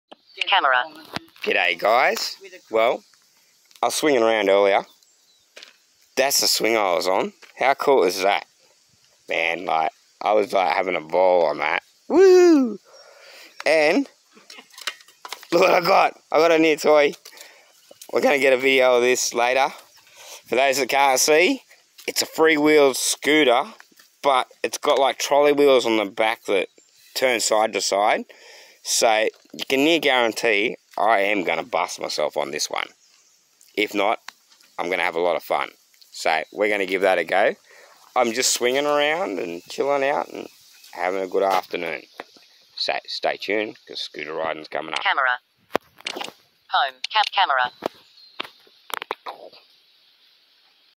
I got a 3 wheel scooter
people give me the strangest things but this one is cool watch how much fun and trouble I get into on this in the next couple of days